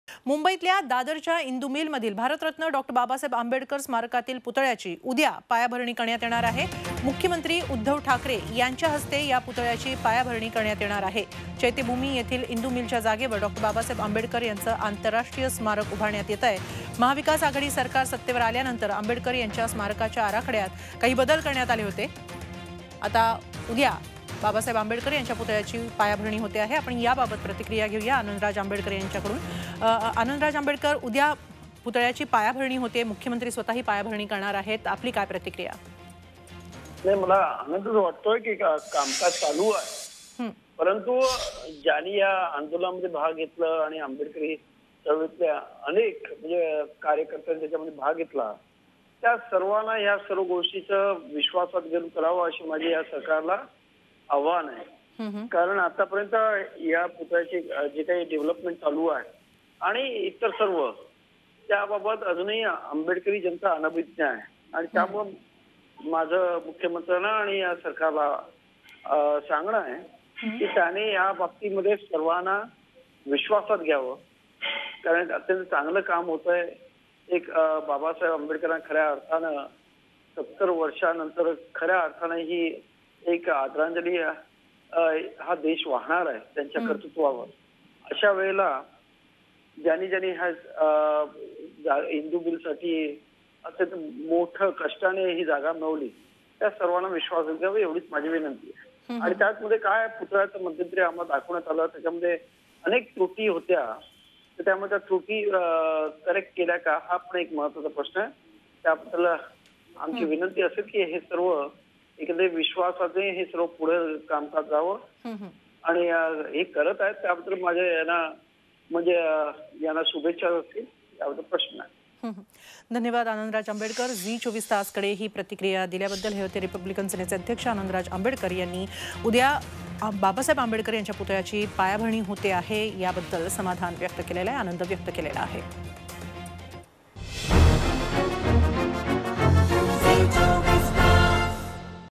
मुंबई | इंदू मिल येथील बाबासाहेब आंबेडकर स्मारकाची उद्या पायाभरणी
Mumbai Foundation Stone Of Dr Babasaheb Ambedkar Statue To Be Laid Tomorrow